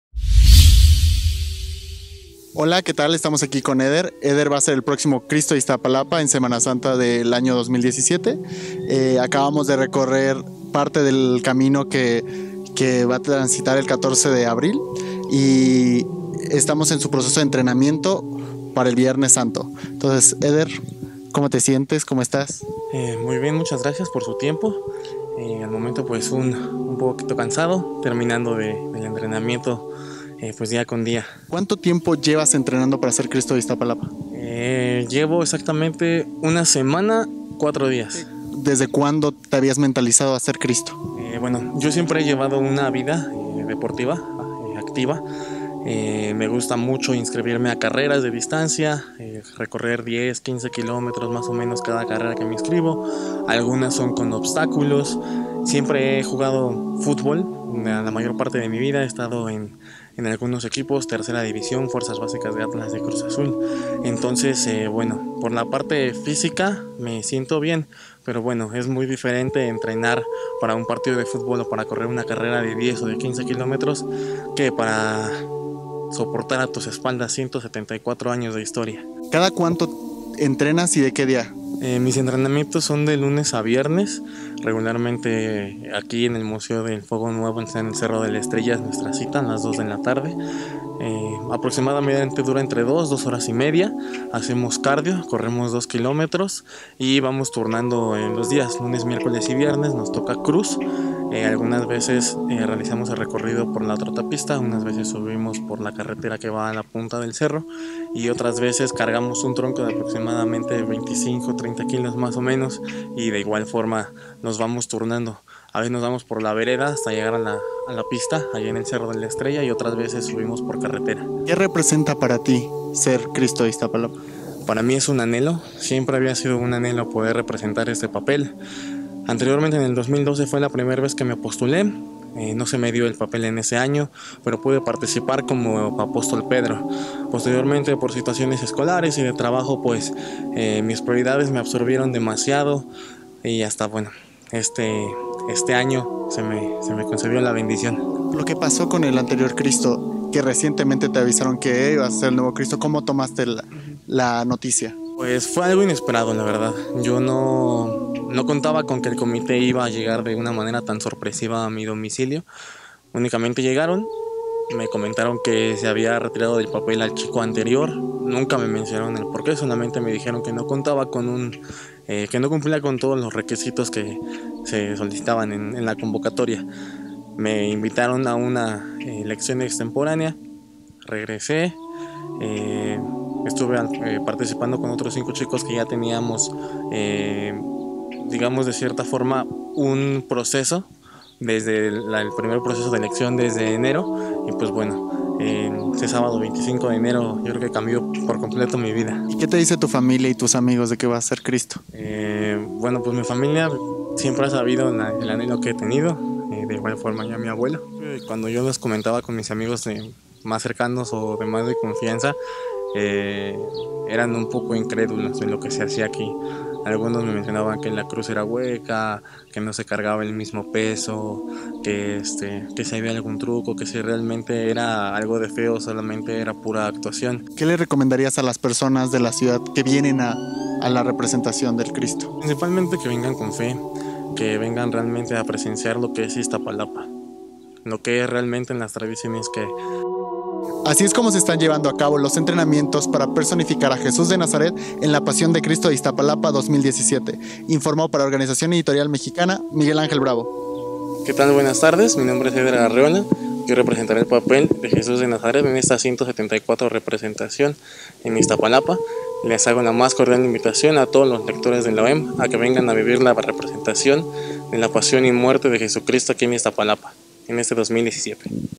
Eder Arreola y la responsabilidad de ser Cristo en la Pasión de Iztapalapa
Eder Arreola tendrá sobre sus hombros el peso de una cruz de 85 kilos, 174 años de tradición y la mirada de más dos millones de personas que presenciarán su crucifixión en la cima del Cerro de la Estrella. Eder personificará a Cristo este año en Iztapalapa.

Lee más: goo.gl/nlg20e